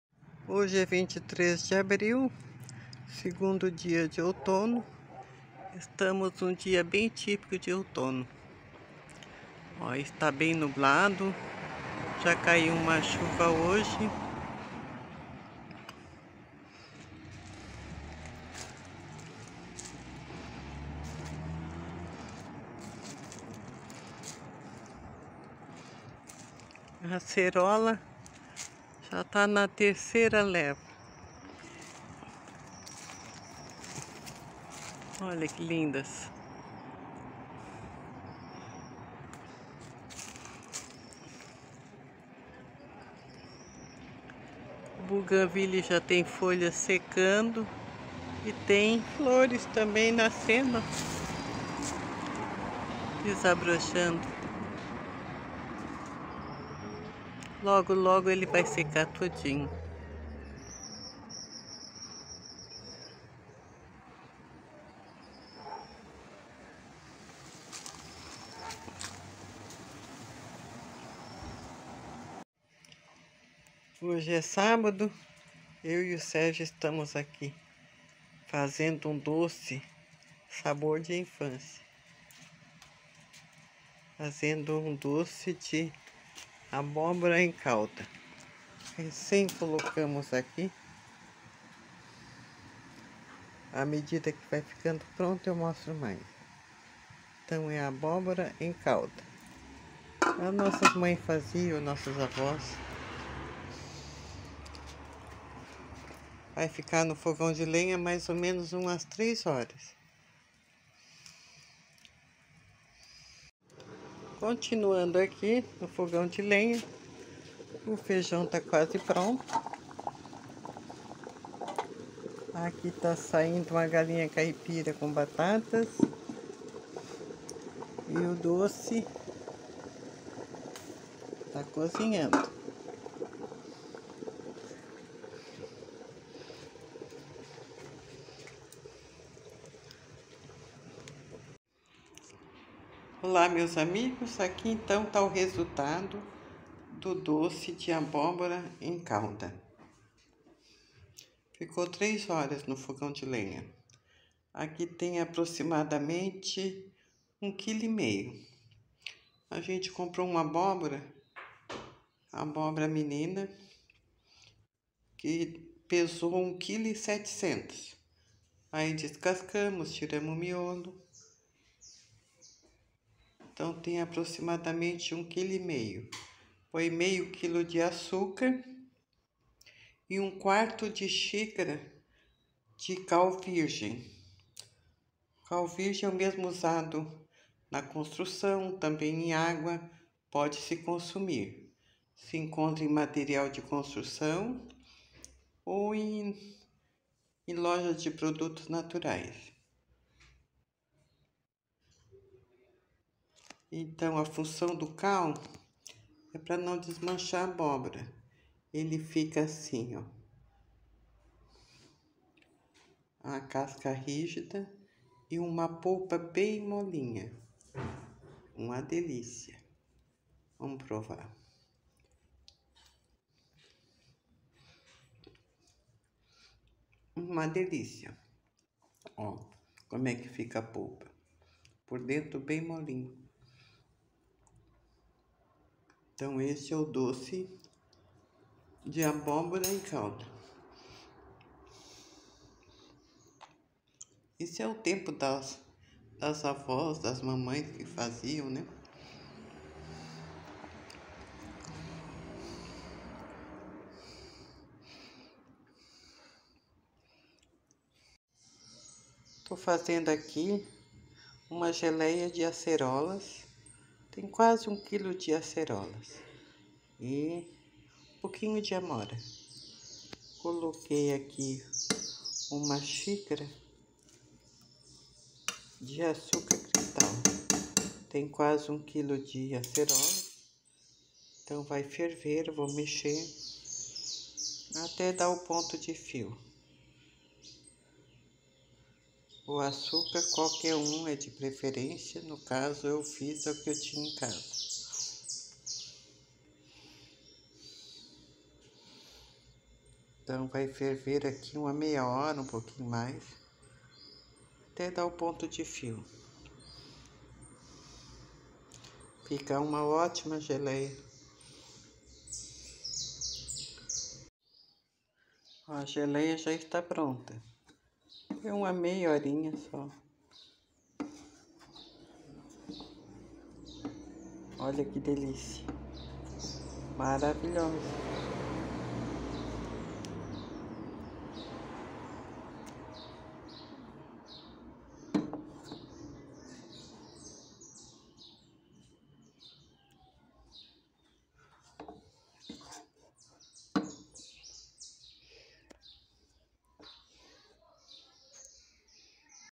Como fiz doce de abóbora em calda e geleia de acerolas
vlog #2024 doce likesharesubscribe Como eu fiz doce de abóbora em calda e geleia de acerolas